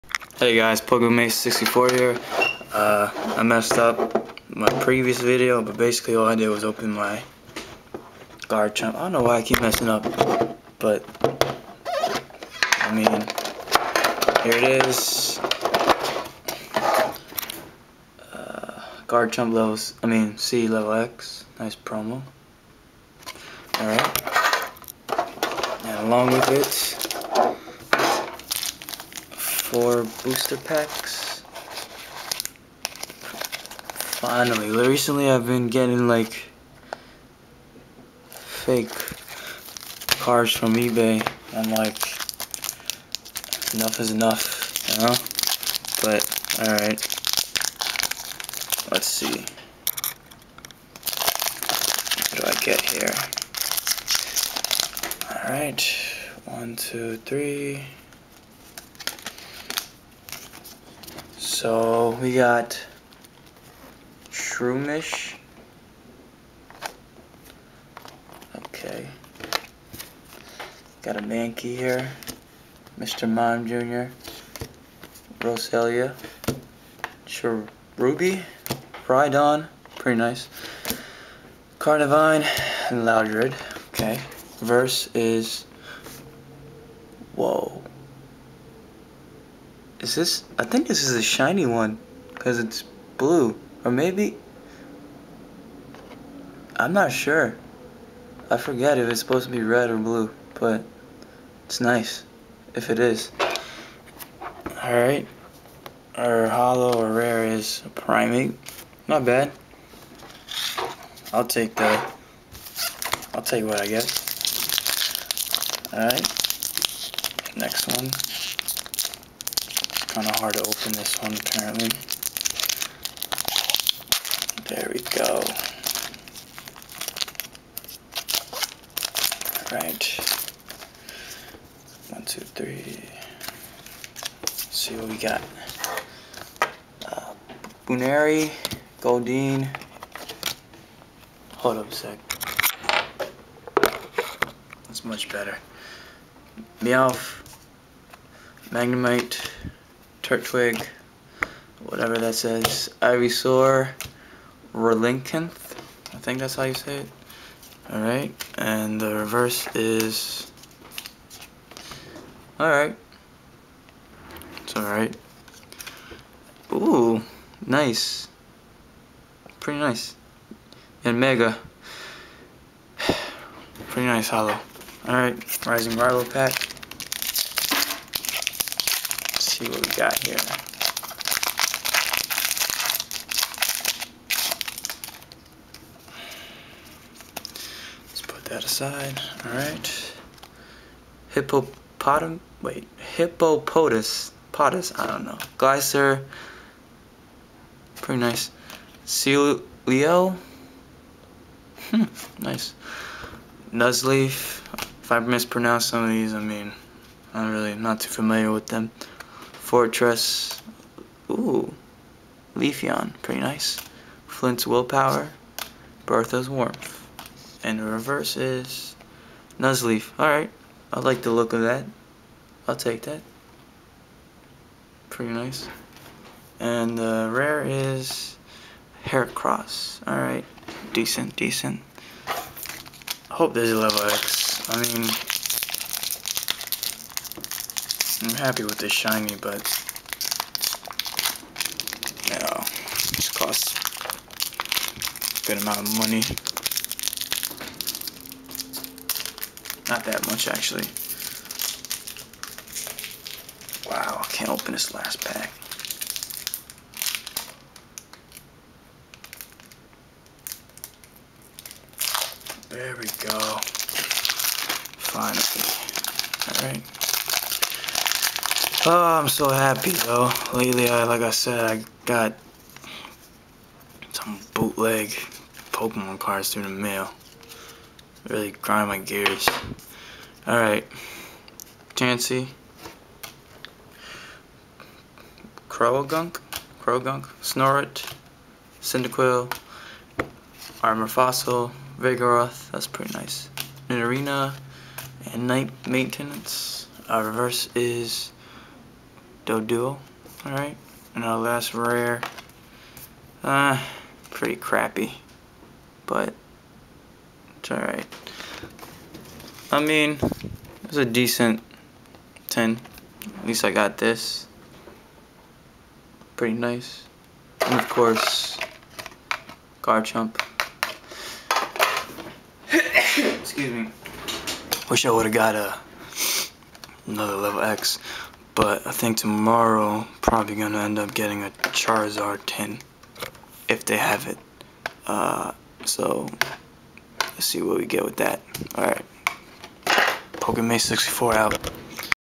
Opening my Garchomp C Lv X Tin (Shiny Milotic Pull!)
not too thrilled from the pulls, but at least i got a shiny milotic!!